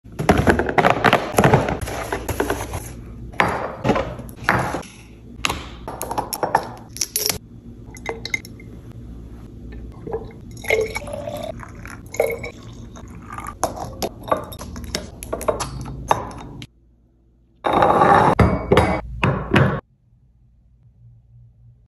ASMR Unboxing Amazon Home Foaming Soap Dispensers | Sturdy Soap Dispensers You Need To Buy
Slowly but surly getting the bathroom together. I love the foam from these sturdy soap dispensers - you NEED to buy!!